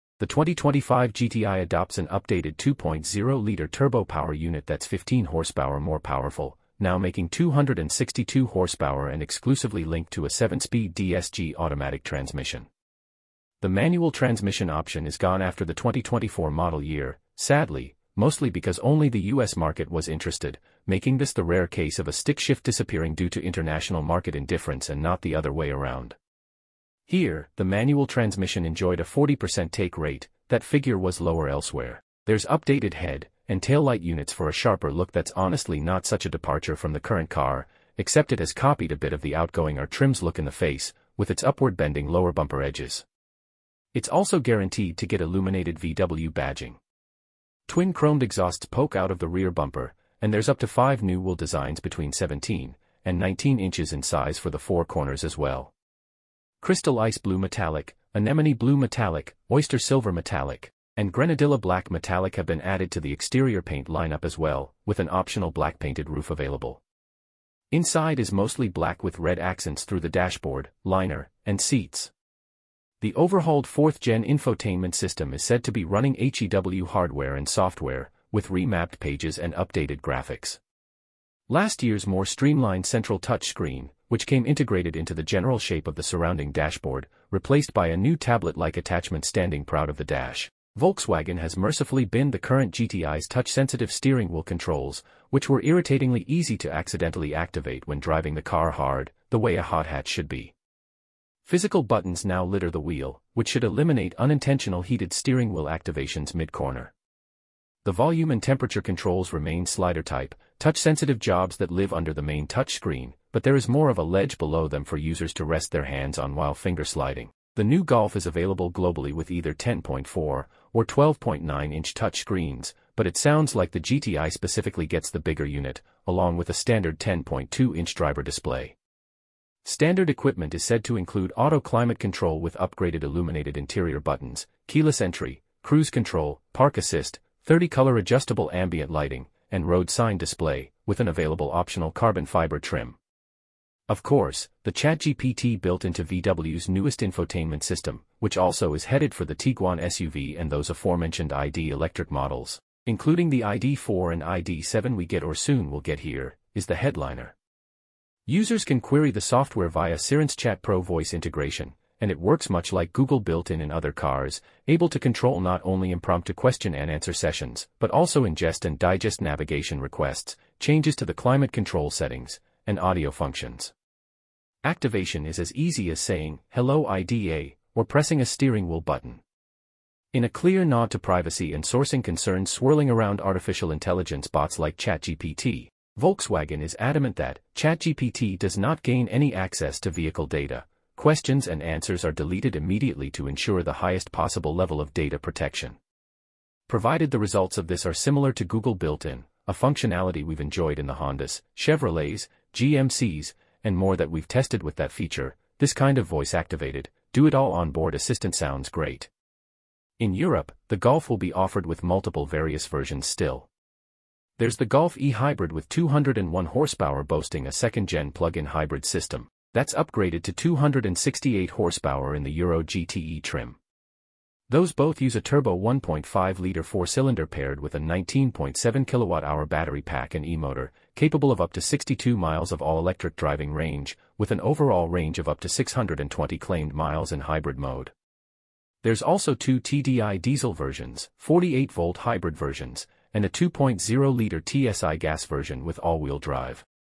2025 Volkswagen Golf GTI.
2025 Volkswagen Golf GTI First Look: More Power, More AI
The ChatGPT AI assistant hops aboard VW's updated hot hatchback.